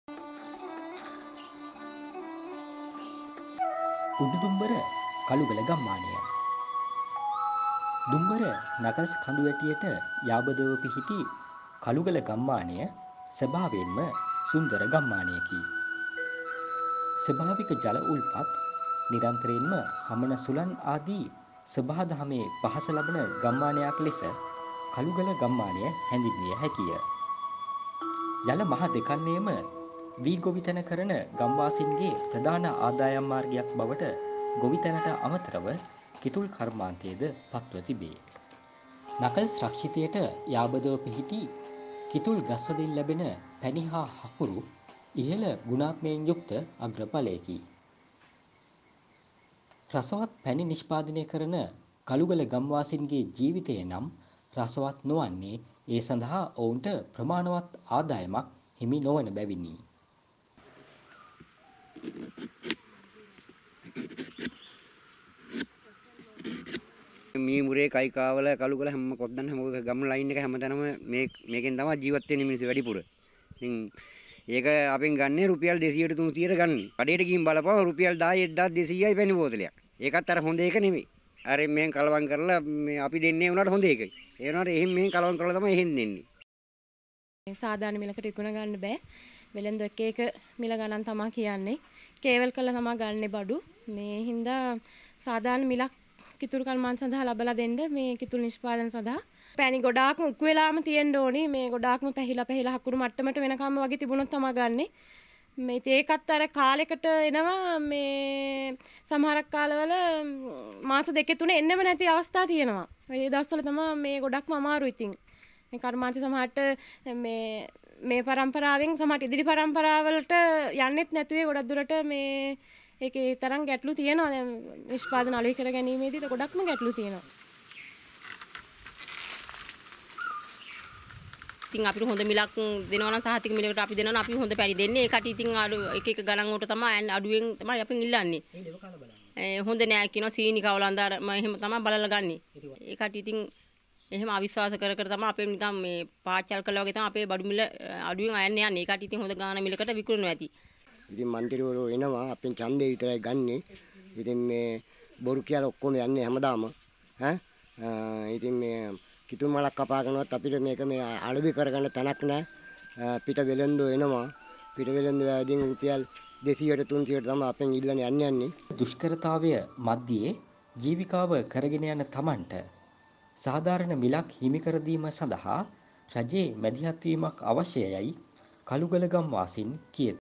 පැණි තරම් රසවත් නොමැති කළුගල කිතුල් කර්මාන්තකරුවන්ගේ කථාව
දුම්බර නකල්ස් රක්ෂිතයට යාබදව පිහිටි කළුගල ගම්මානය ප්‍රකට වන්නේ කිතුල් පැණි හා හකුරු නිෂ්පාදනය සම්බන්ධයෙනි. වසර ගණනාවක් තිස්සේ කරන එම කර්මාන්තයට අද වන විට අත්වී ඇත්තේ ඛේදනීය ඉරණමකි.

Around the Kandy
මහනුවර හා ඒ අවට සිදුවීම් සැකෙවින්...